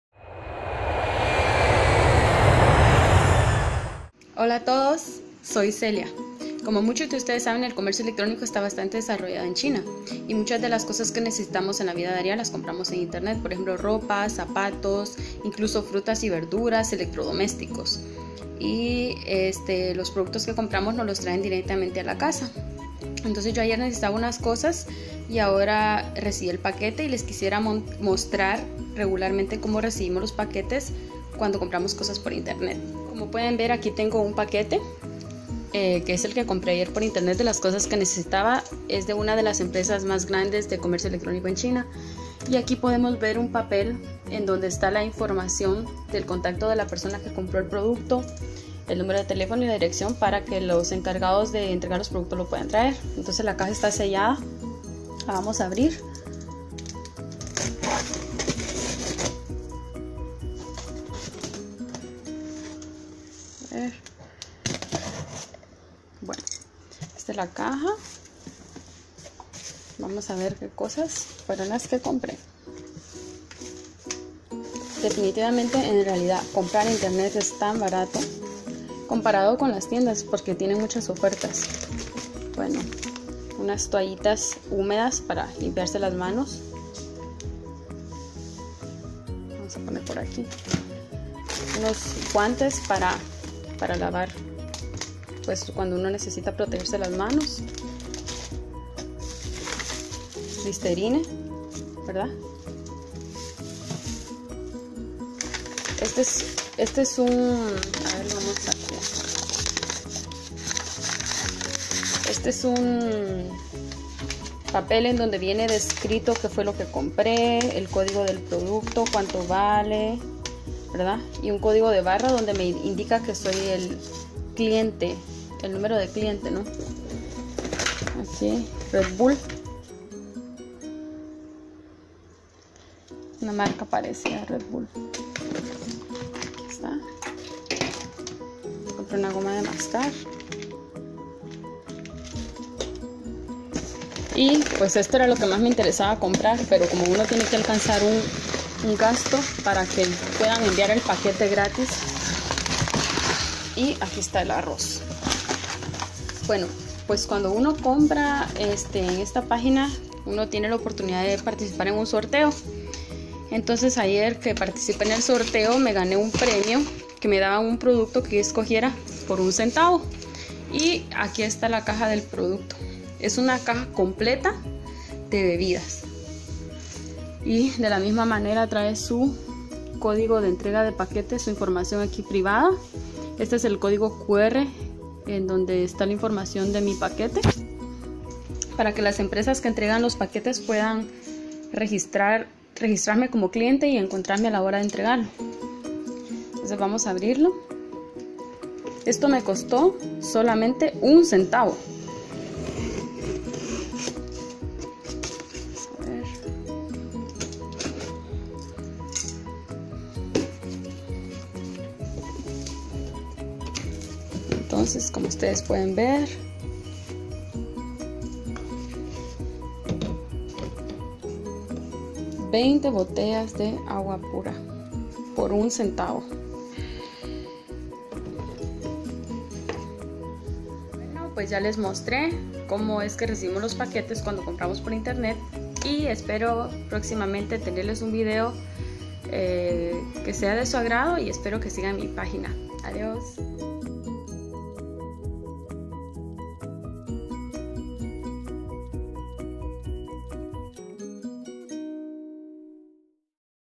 ¿Qué podemos comprar por medio de internet en China? PARTE 1
El presente video muestra algunos de los productos que podemos comprar en internet en China. Además, podemos ver cómo se reciben los paquetes de las compras hechas por internet.